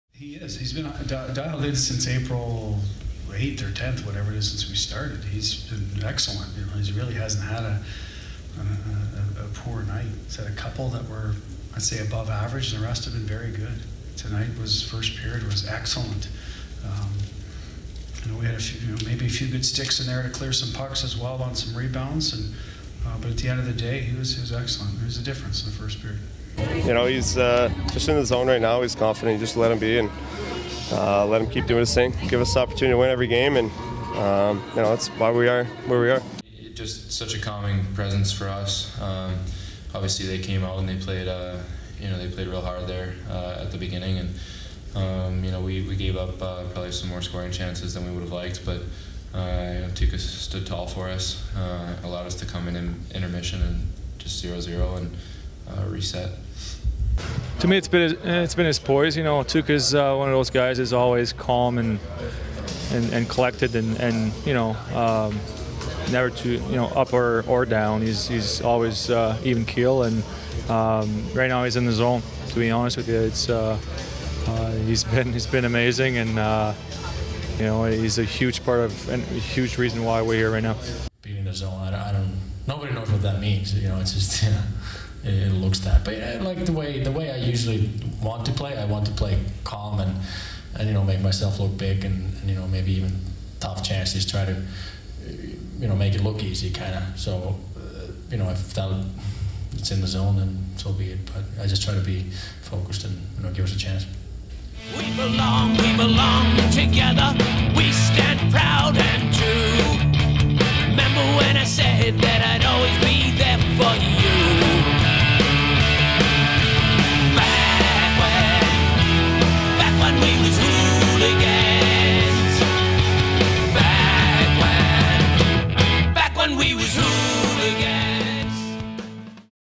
Tuukka Rask backstops the Bruins within a game of the Cup Finals
Where would the Bruins be without Tuukka Rask?

Follow us on all of social media platforms"